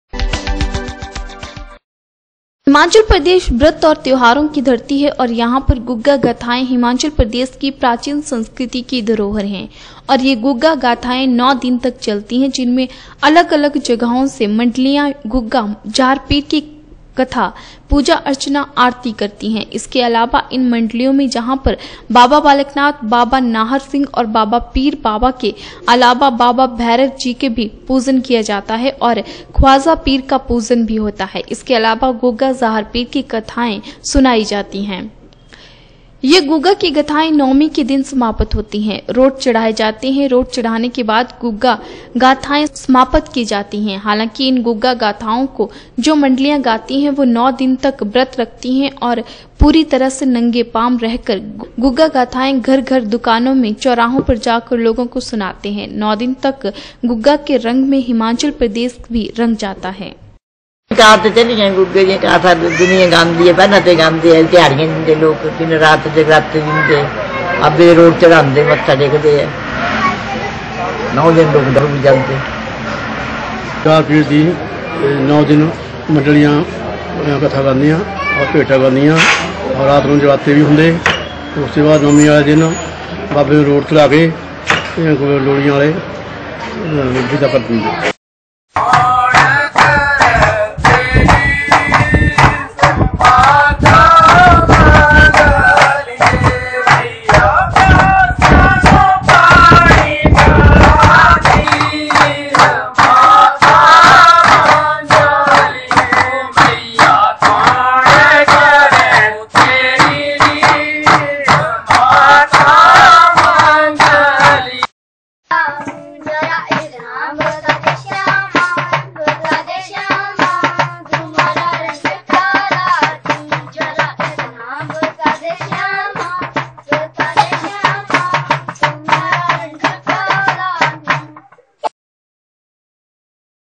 बाबा बालक नाथ ण्बाबा नाहर सिंह और बाबा पीर बाबा के अलावा बाबा भैरव जी का भी पूजन किया जाता
हिमाचल प्रदेश व्रत और त्यौहारों की धरती है और यहां पर गुगा  गाथाएं हिमाचल प्रदेश की प्राचीन संस्कृति की धरोहर है और यह गुगा  गाथाएं 9 दिन तक चलती है जिसमें अलग.अलग जगह से मंडलियां गोगा जाहरपीर की कथा पूजा अर्चना आरती करती है इसके अलावा इन मंडलियों में जहां पर बाबा बालक नाथ ण्बाबा नाहर सिंह और बाबा पीर बाबा के अलावा बाबा भैरव जी का भी पूजन किया जाता है ओर ख्वाजा पीर का पूजन होता है इसके अलावा गुगा जाहर पीर की कथाएं  गाई जाती है